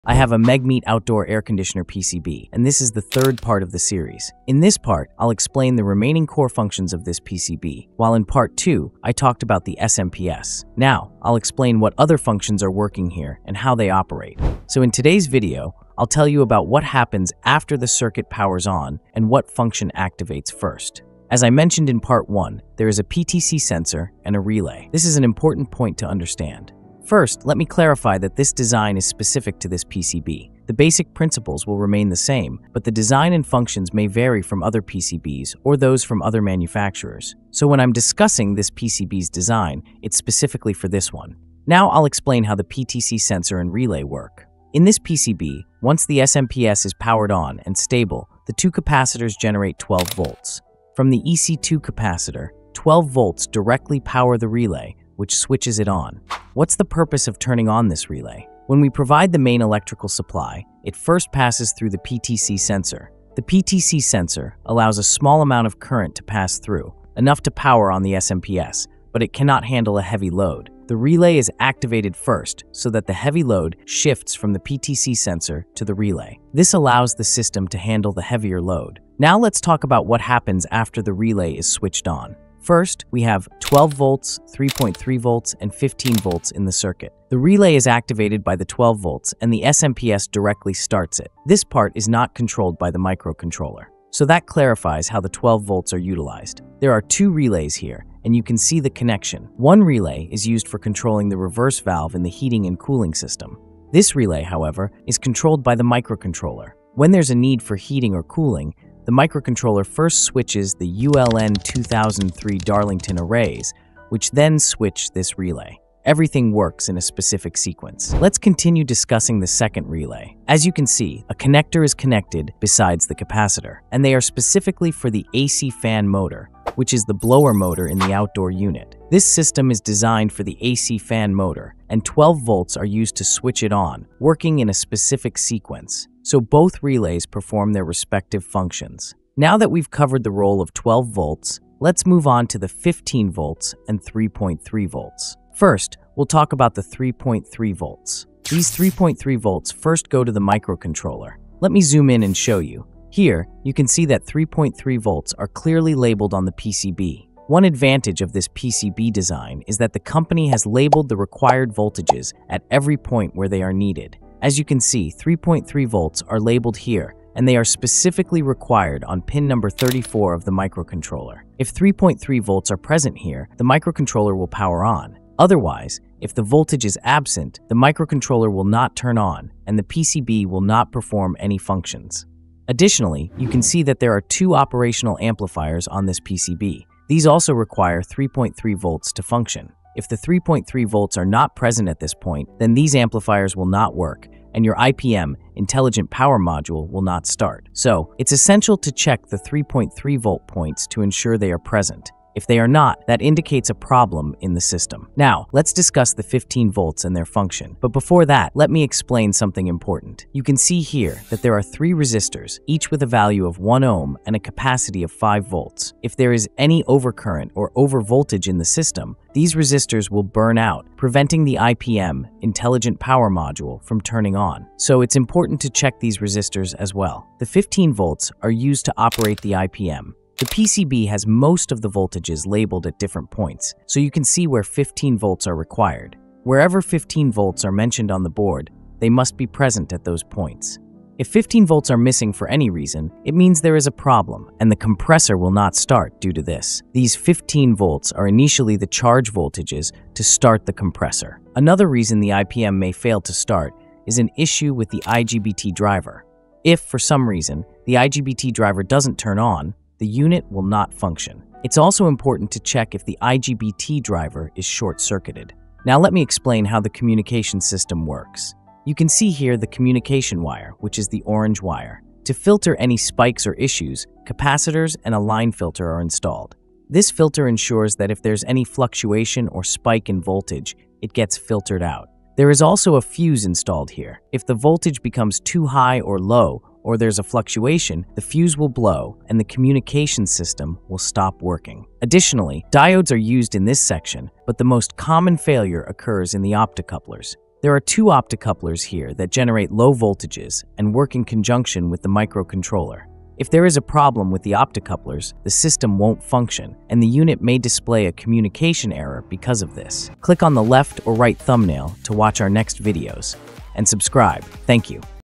Critical Voltages in Megmeet AC Circuit: 12V, 3.3V, 15V Explained!
This video explores the critical roles of 12V, 3.3V, and 15V in Megmeet mini split air conditioner circuits and why these voltages are essential for efficient operation. I’ll explain how these voltages power key components like the microcontroller, relays, and the IPM (Intelligent Power Module). Whether you're troubleshooting voltage issues or just curious about how mini-split AC circuits work, this video breaks down each voltage’s function and the common issues that can arise if they fail. If you want to deepen your understanding of mini-split AC circuit boards and improve your repair skills, this video has you covered. Watch now to learn more about the backbone of your mini split air conditioner’s electrical system!